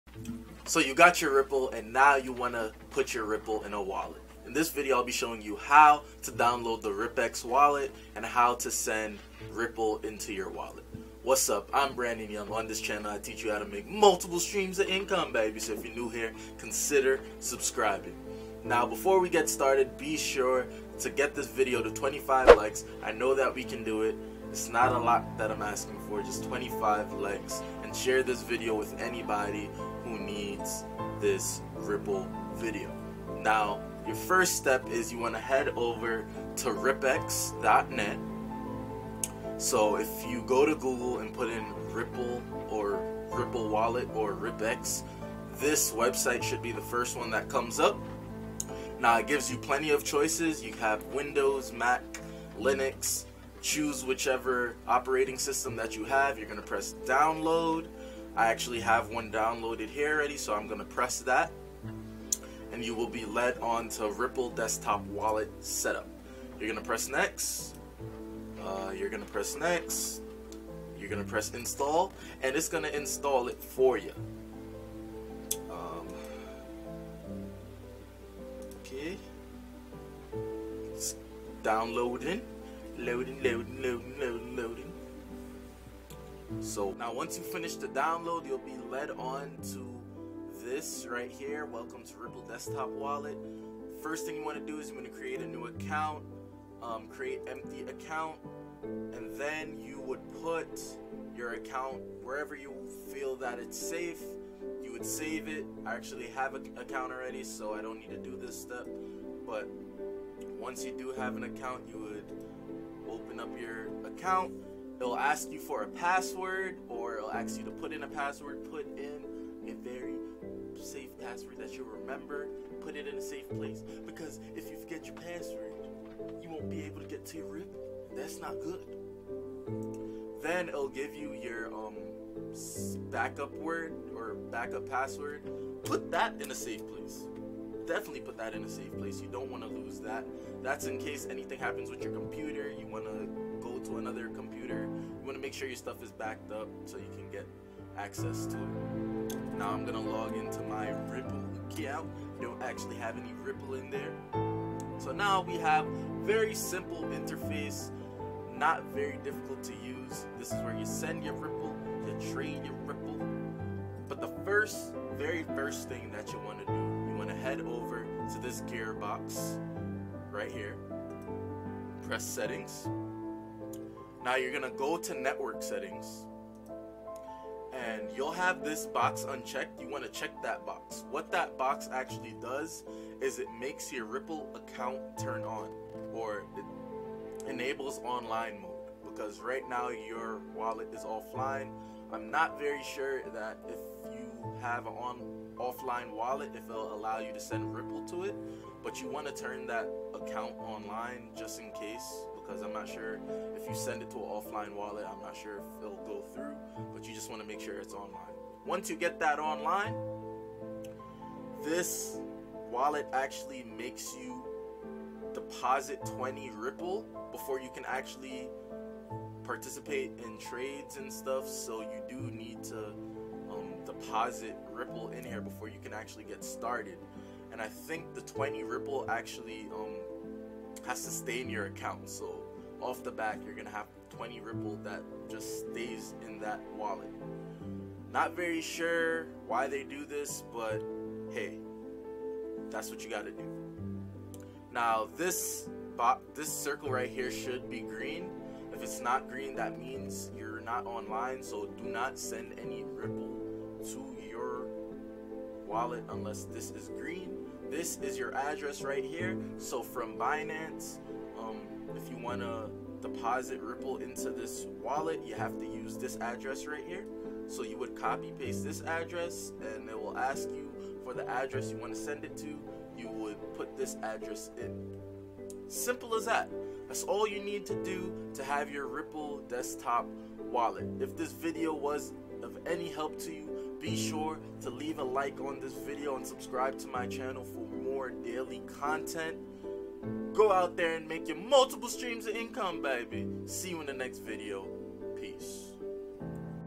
HOW TO SETUP RIPPLE DESKTOP WALLET - RIPPEX TUTORIAL !
I show you how to setup RIPPEX which is a ripple desktop wallet!

Canon T5i

GO READ MY BLOGS!